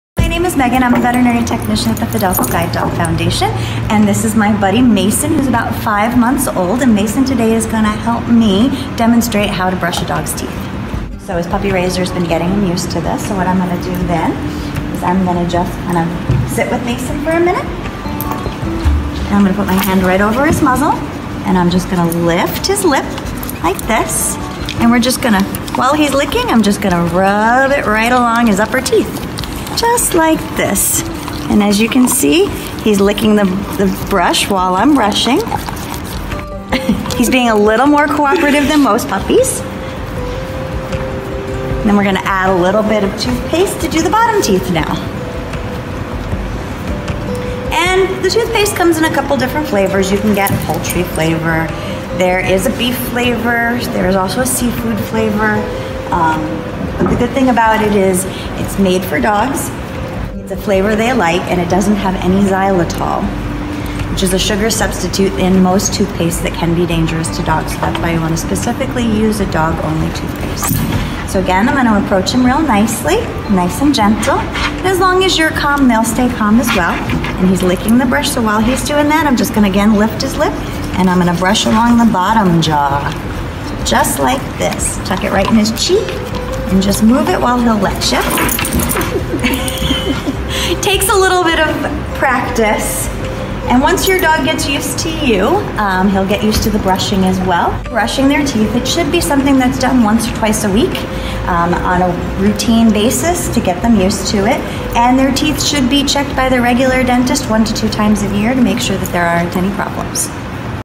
February is Pet Dental Health Month!
Did you know that February is pet dental health month and Megan & "Mason" are here to show you the best way to clean those pearly whites!

VD: Vet tech Megan and 5-month-old pup Mason explain how to clean your pups teeth.